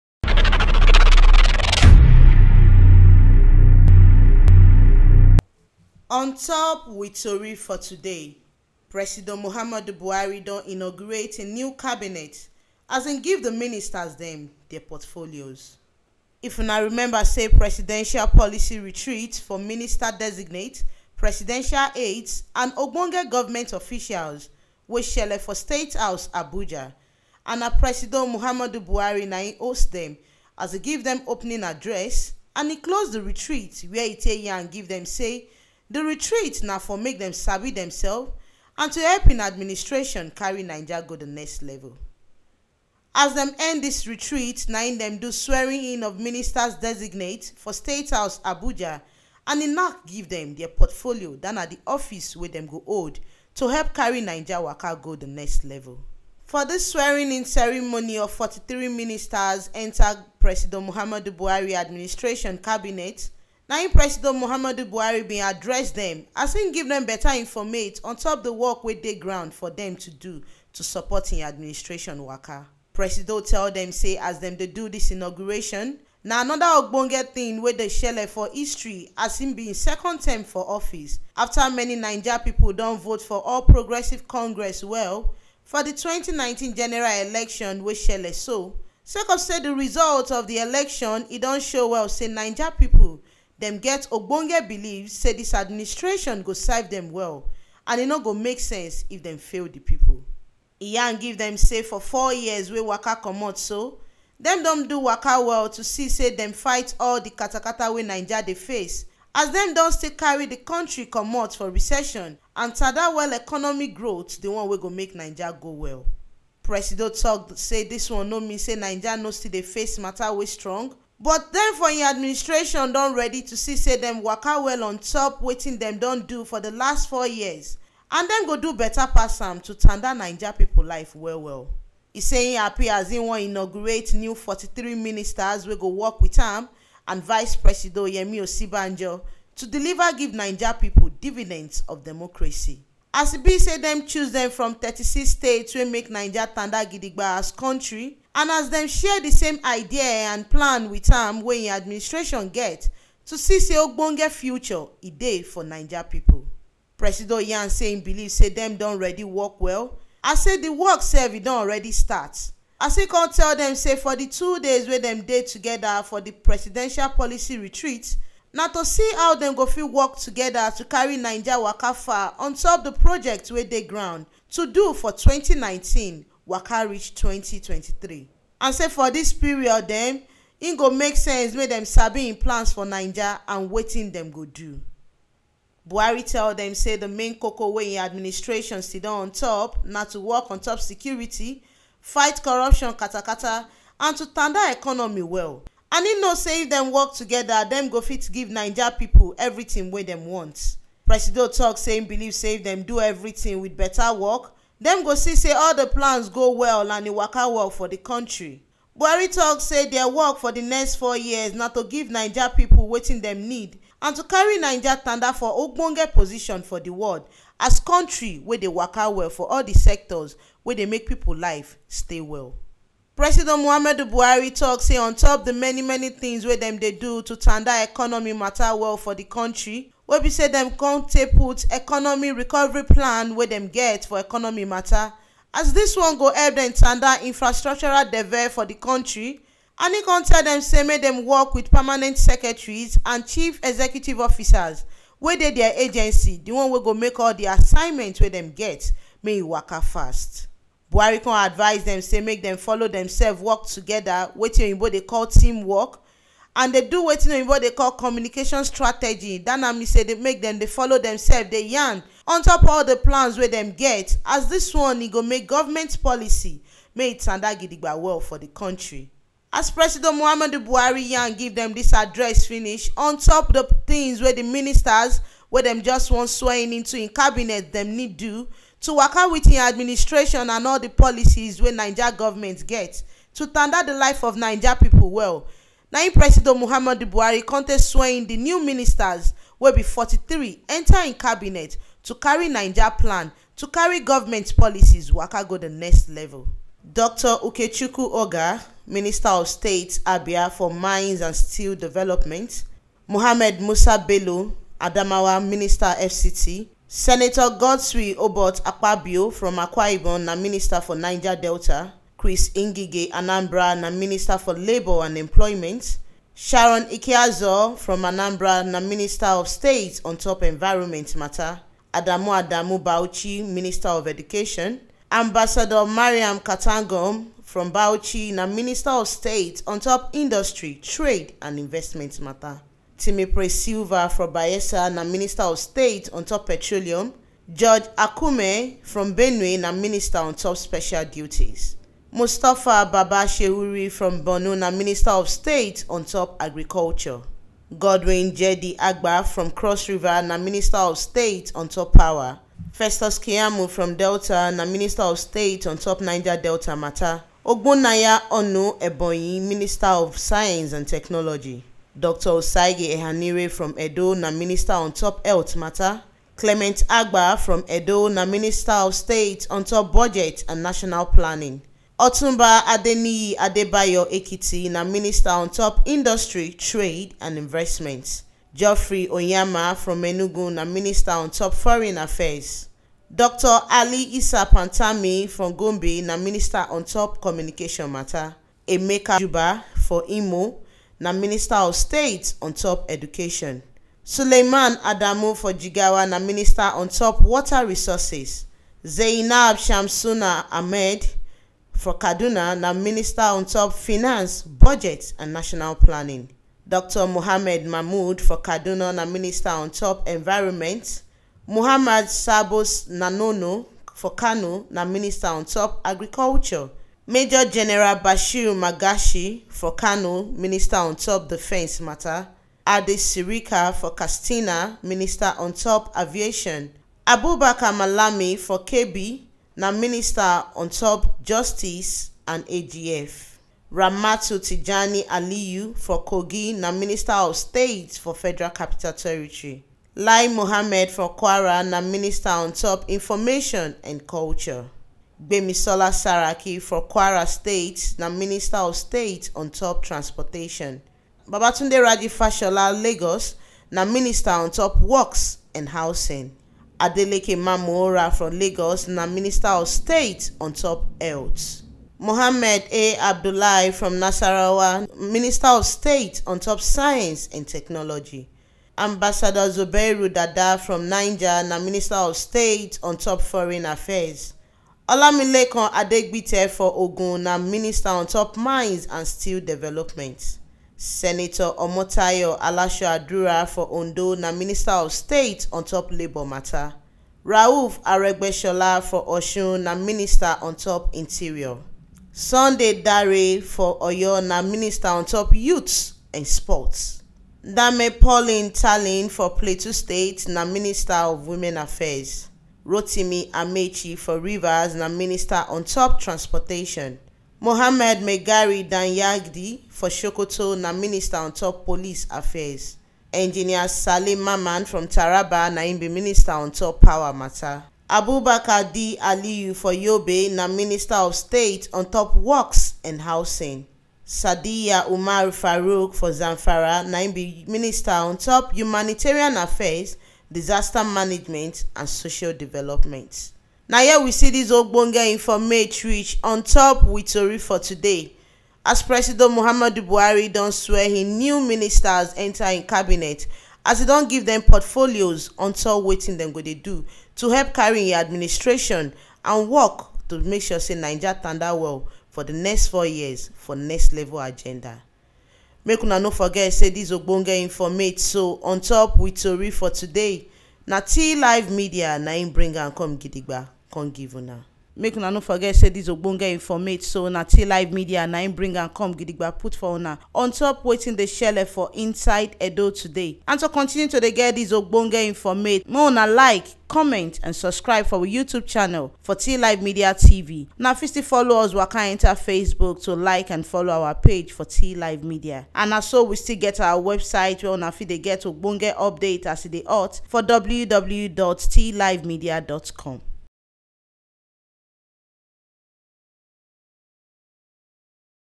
Presido Buhari Don Swear In New Ministers, As E Knack Dem Deir Portfolios
log onto to dey get more ogbonge updates